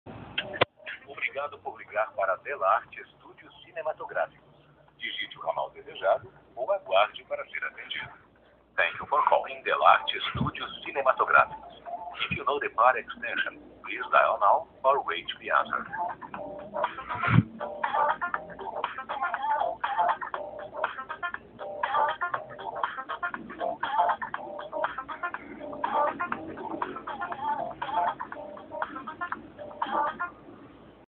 Música de espera/mensagem da Delart
Música de espera/mensagem feita em português e inglês da Delart, o maior estúdio de dublagem do Rio de Janeiro, com locução de Luiz Feier Motta, locutor da Delart